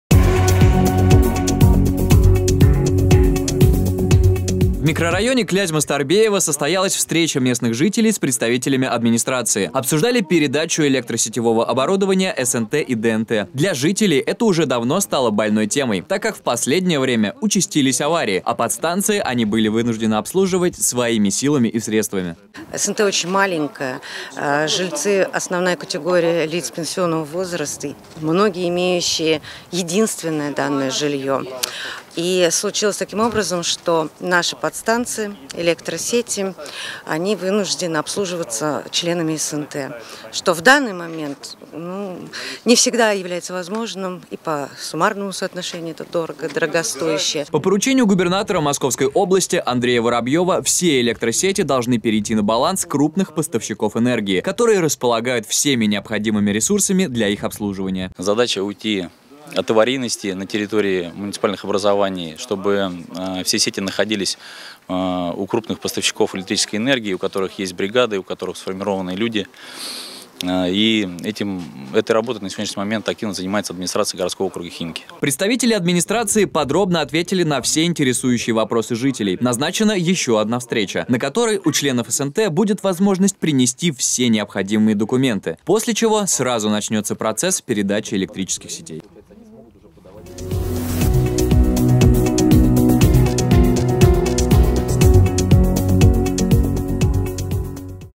Обслуживание электросетей поручат крупным компаниям. 23.03.2022
В микрорайоне Клязьма-Старбеево состоялась встреча местных жителей с представителями администрации. Обсуждали передачу электросетевого оборудования СНТ и ДНТ. Для жителей это уже давно стало больной темой, так как в последнее время участились аварии, а подстанции они были вынуждены обслуживать своими силами и средствами.